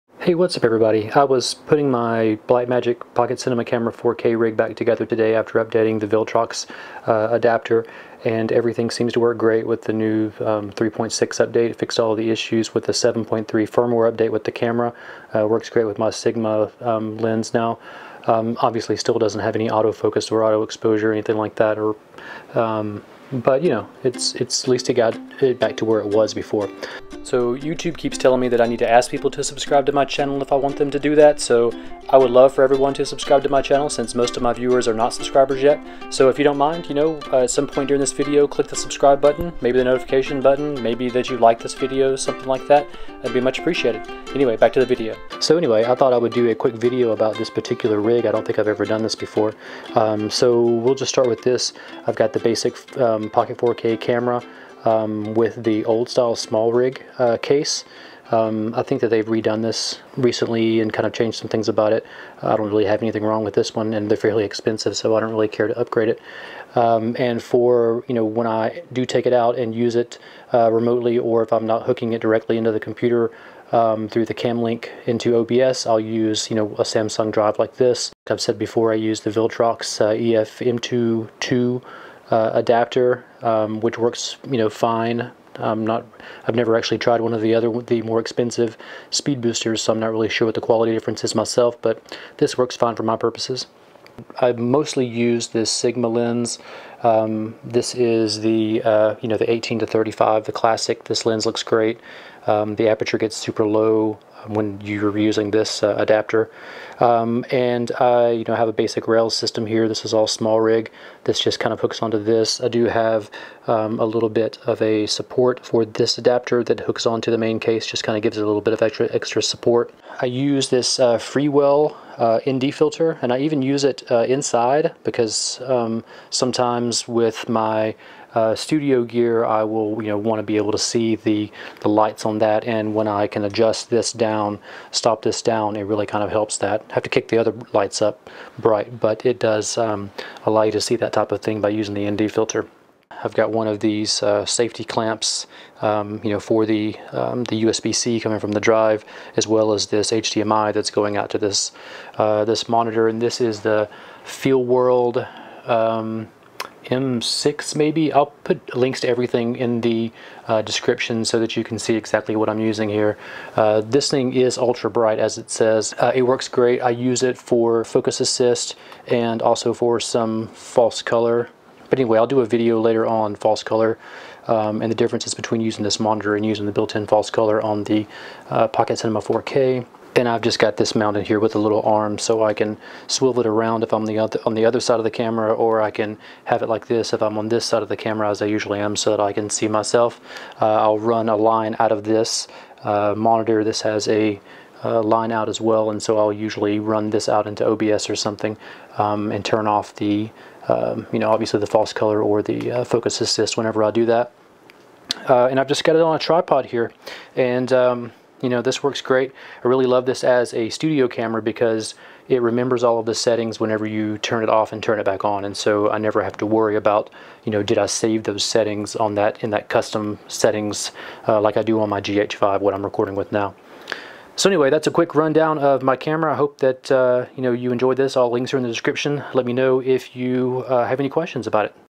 MY BMPCC4k RIG 2021 - VILTROX - SIGMA 18-35 - SMALLRIG
I've never really done a rig video before, so thought now is a good time since lots of my subscribers watch the BMPCC4k videos.

Cameras:
Panasonic GH5
Black Magic Pocket Cinema Camera 4K
Insta360 One X
DJI Osmo Pocket
DJI Osmo Action
iPhone 11 Pro Max
Lumix GX-7 (newer model)